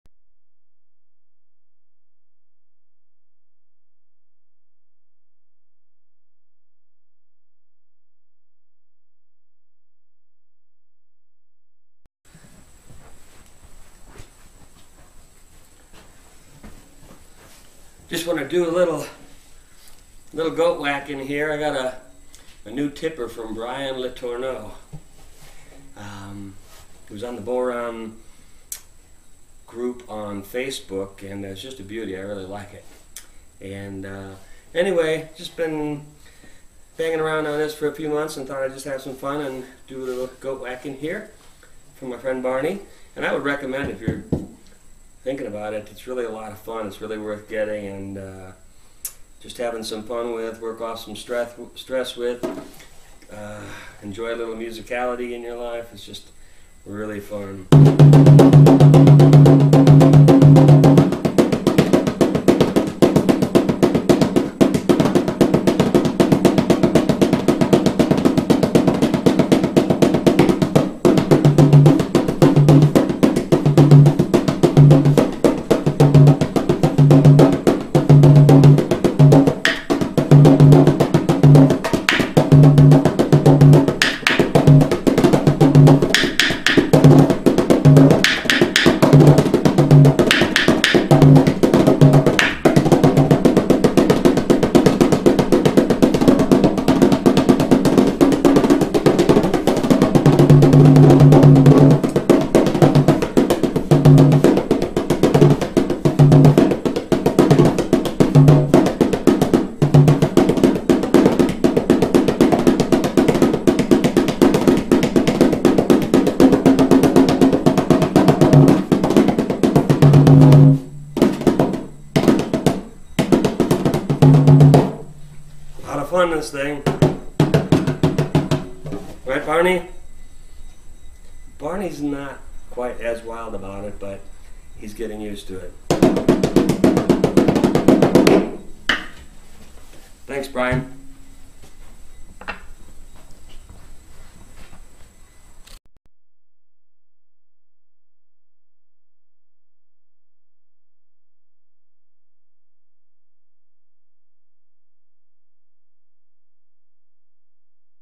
Whacking The Goat with one of Brian's Beaters
A little friendly, if unskilled, whacking of the afternoon goat, with a beater/tipper from Brian LeTorneau...it is a beauty and you should look him up at Brian's Beaters.